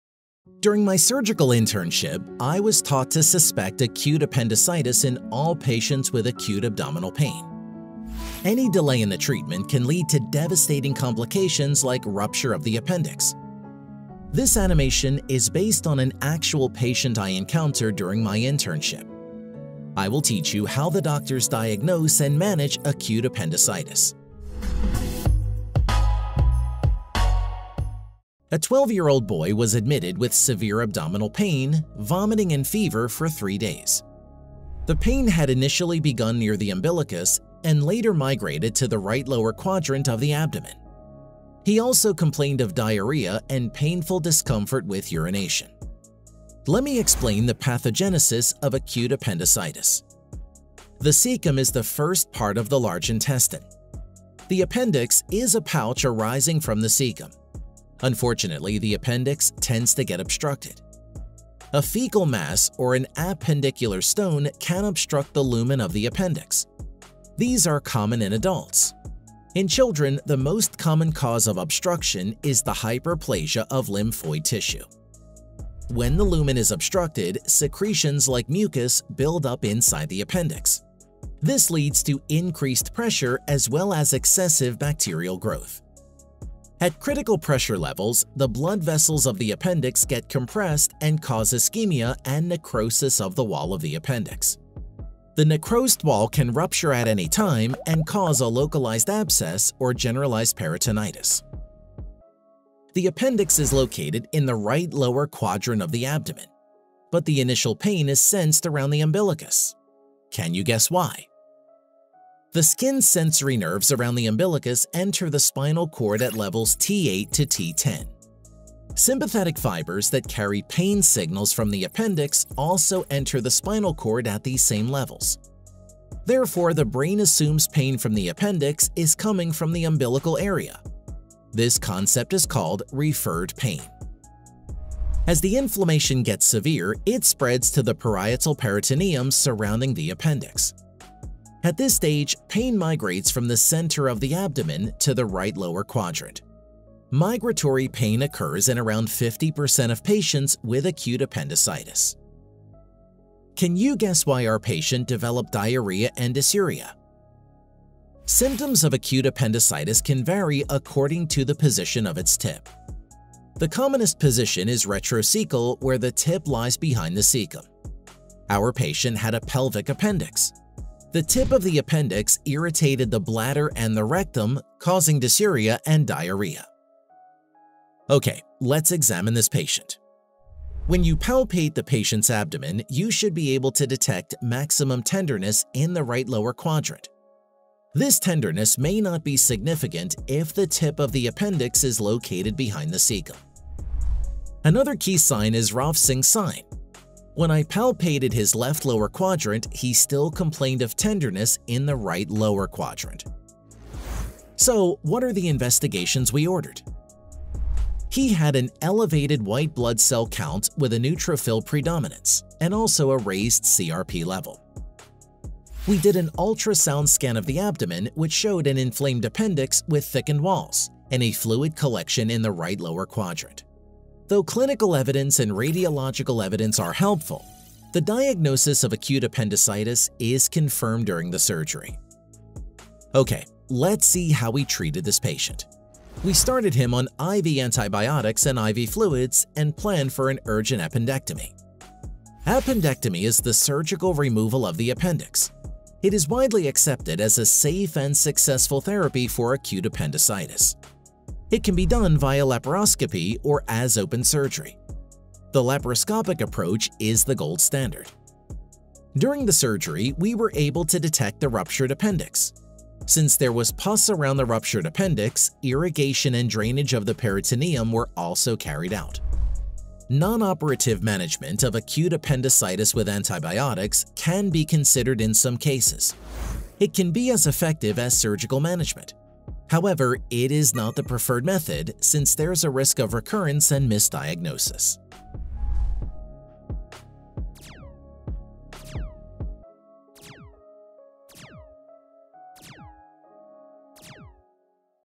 What is Appendicitis? A real case of Burst Appendix.
In this video, I’m going to teach you about a complicated case of acute appendicitis. What can happen when the Appendix ruptures? It is a life-threatening surgical emergency.

First, I’ll show you how and why the appendix gets inflamed. Then I’ll show you why it can get ruptured.

Next, I’ll briefly explain the symptoms and the pathophysiology, including the referred pain seen in Acute appendicitis.

Some patients develop Diarrhea and Dysuria with Appendicitis. Don’t worry! You’ll learn the exact causes for those symptoms as well.

Then, I’ll show you How doctors examine patients with appendicitis, and the investigations they order to diagnose the condition.

Finally, I’ll teach you the management options, such as antibiotics therapy, and surgical options, including open vs. laparoscopic appendectomies.